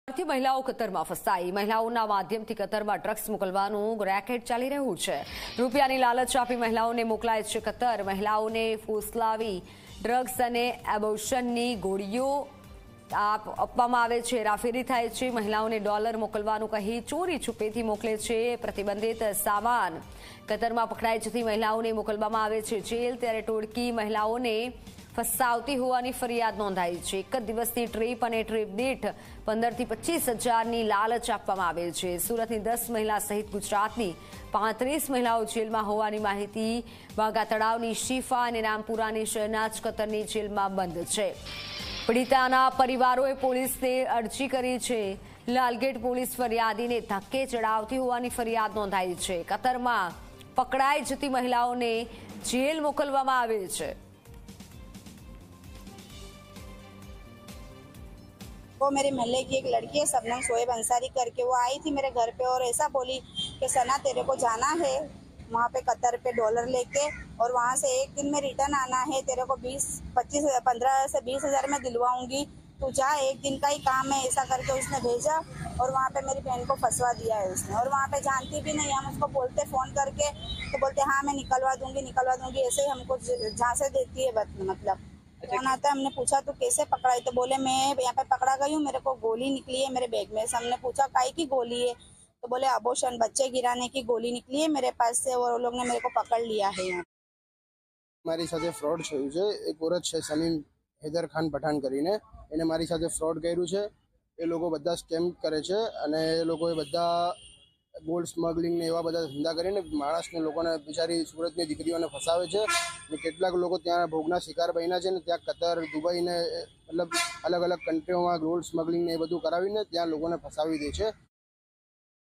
Several women from Surat trapped in Qatar, being allegedly forced to smuggle prohibited materials
સુરતની ભારતીય મહિલાઓ કતરમાં ફસાઈ---

મહિલાઓના માધ્યમથી કતરમાં ડ્રગ્સ મોકલવાનું ચાલી રહ્યું છે રેકેટ---

રૂપિયાની લાલચ આપી મહિલાઓને મોકલાય છે કતર--

મહિલાઓને ફોસલાવી ડ્રગ્સ અને અબોર્શનની ગોળીની થાય છે હેરાફેરી--

મહિલાઓને ડૉલર મોકલવાનું કહી ચોરી છુપીથી મોકલે છે પ્રતિબંધિત સામાન--

કતરમાં પકડાઈ જતી મહિલાઓને મોકલવામાં આવે છે જેલ--

ટોળકી મહિલાઓને ફસાવતી હોવાની કરાઈ ફરિયાદ--

એક જ દિવસની ટ્રિપ અને ટ્રિપ દીઠ 15 થી 25 હજારની આપવામાં આવે છે લાલચ--

સુરતની 10 સહિત ગુજરાતની 35 મહિલાઓ જેલમાં હોવાની માહિતી--

ભાગાતળાવની શીફા અને રામપુરાની શહેનાઝ કતરની જેલમાં બંધ--

પીડિતાના પરિવારોએ પોલીસને કરી અરજી--

લાલગેટ પોલીસ ફરિયાદીને ધક્કે ચડાવતી હોવાની ફરિયાદ